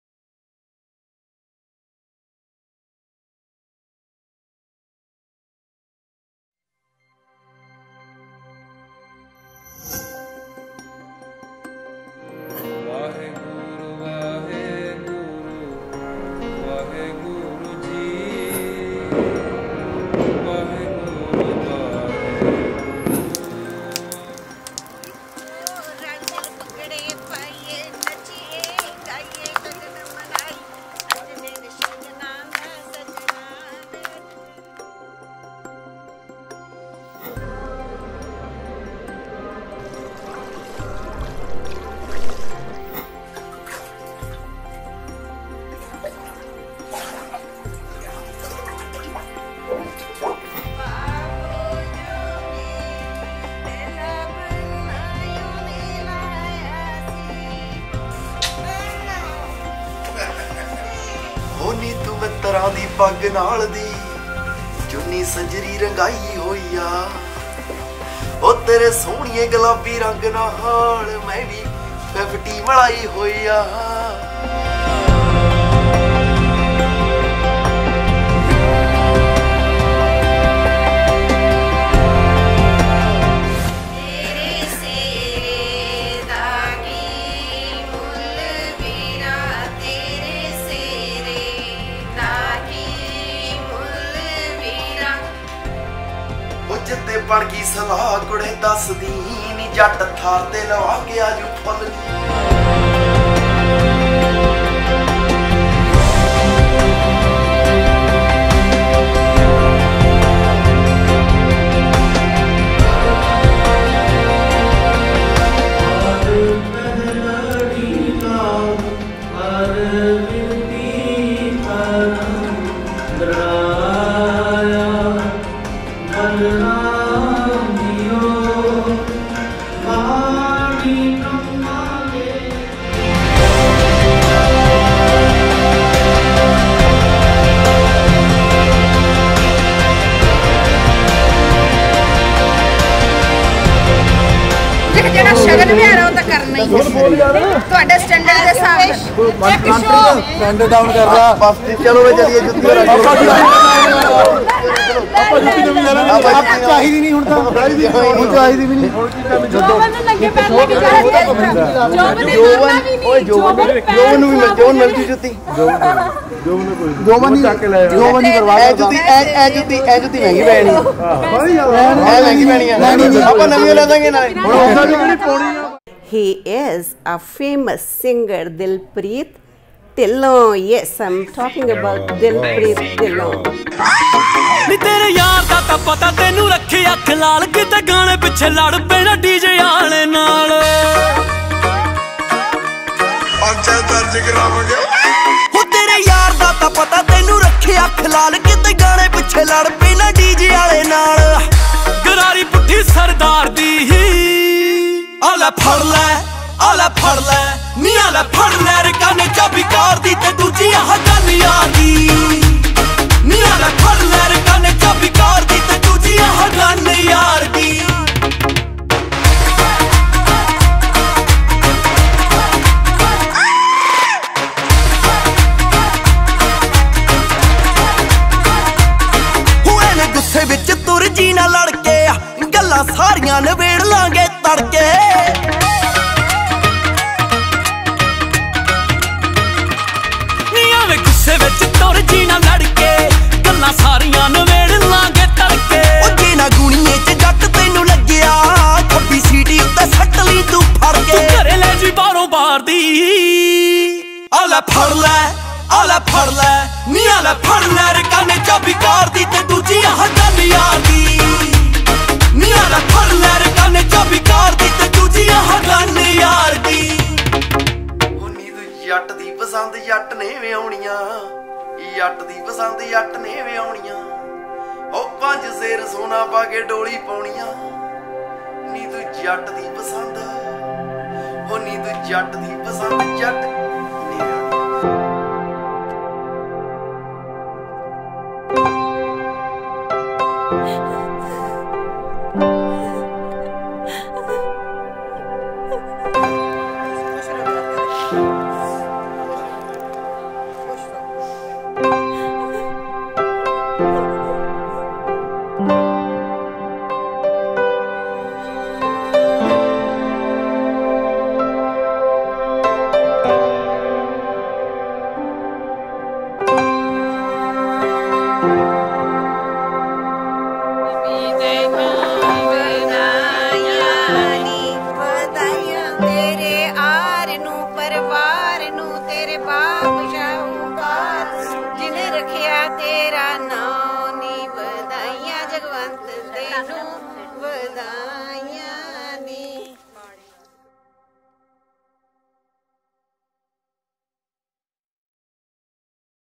Dilpreet Dhillon + Aamber | Wedding Day | A film by Mehar | Mehar Photography | 2018
Dilpreet dhillon weds Aamber dhaliwal | Chandigarh 2018.

Beautiful couple got married in chandigarh it was fun working with them.. sharing a beautiful film of their wedding day with you all here, i hope you all will love it.

if you like it do share it on your social media profiles

Mehar.

Thankyou!